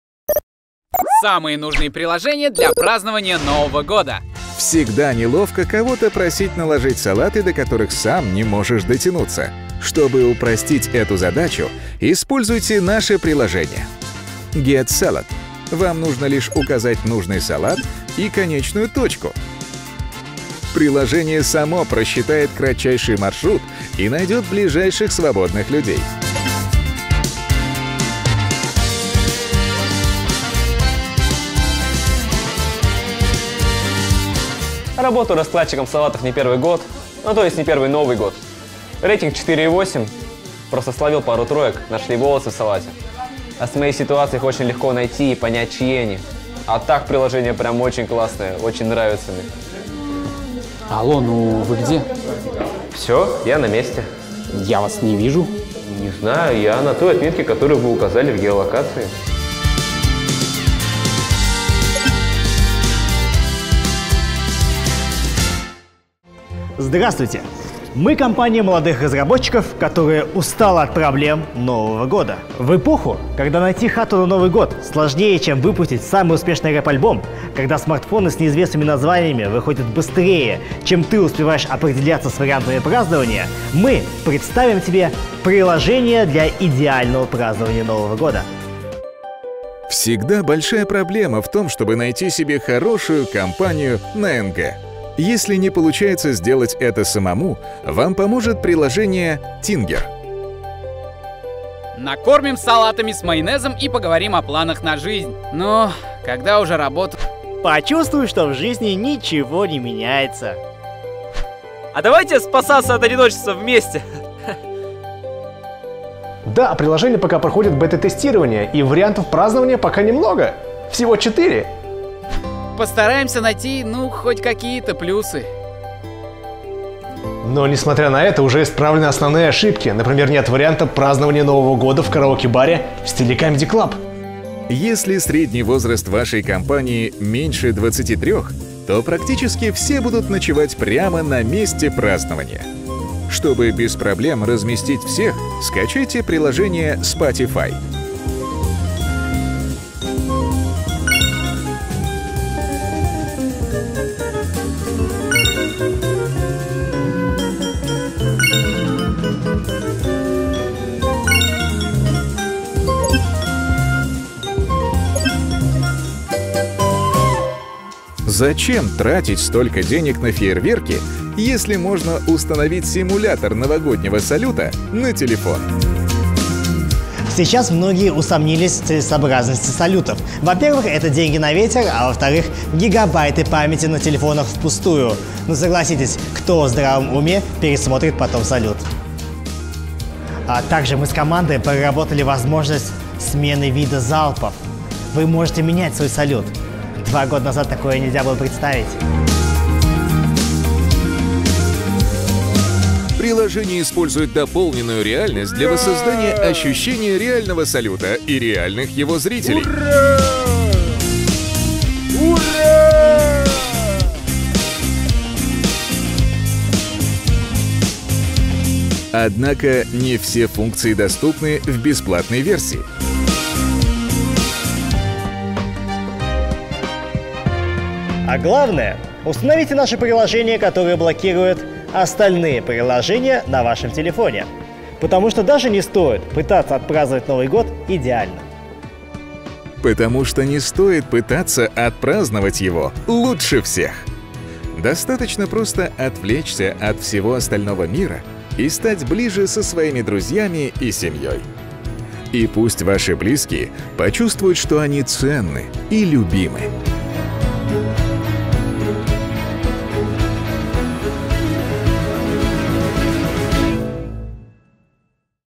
САМЫЕ НУЖНЫЕ ПРИЛОЖЕНИЯ ДЛЯ НОВОГО ГОДА | Громкие рыбы
Если бы эти приложения реально существовали, они облегчили бы жизнь всех во время празднования Нового года.

Съемка и монтаж: Тимофей Шарагин
Моушн дизайн: Михаил Гиберт